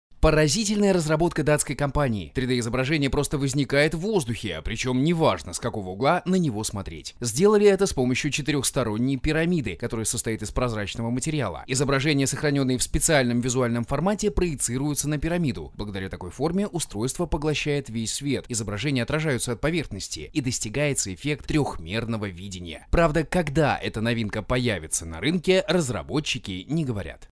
Инфографика. 3D голограмма.
24/08/2010
Инфографика

Поразительная разработка датской компании! 3D изображение просто возникает в воздухе. При чем не важно, с какого угла на него смотреть! Сделали это с помощью четырехсторонней пирамиды, которая состоит из прозрачного материала. Изображения, сохраненные в специальном визуальном формате, проецируются на пирамиду. Благодаря такой форме, устройство поглощает весь свет, изображения отражаются от поверхности, и достигается эффект трехмерного видения. Правда, когда эта новинка пояится на рынке, разработчики не говорят!